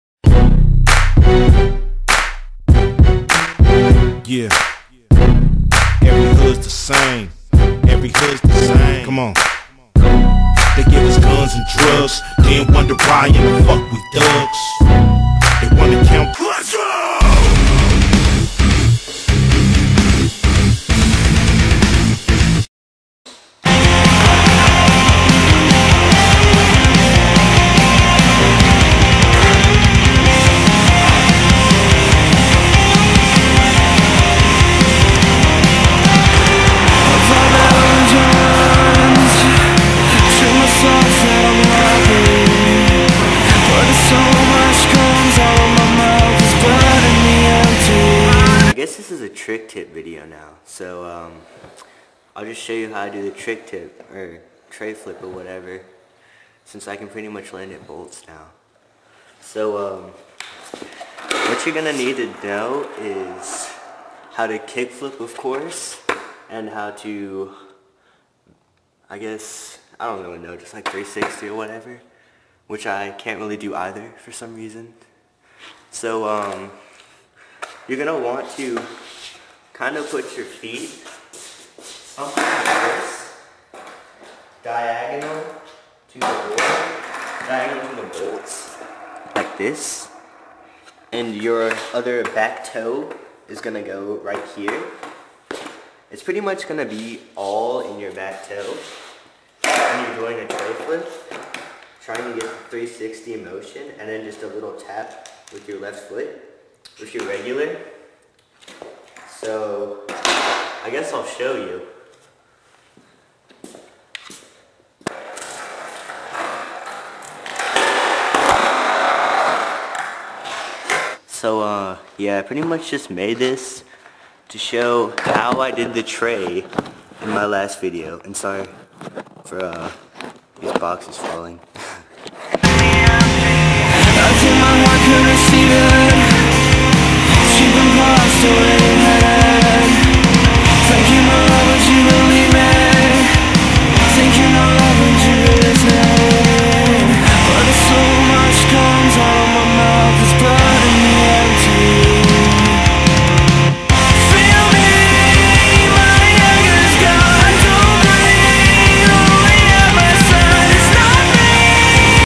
How to Tre Flip - Sk8cheeze - Video Response
Thanks to libby for nothing. AND SORRY I DIDNT PLAN THIS SO I DIDNT KNOW WHAT TO SAY

Rye shows you how to Tre flip!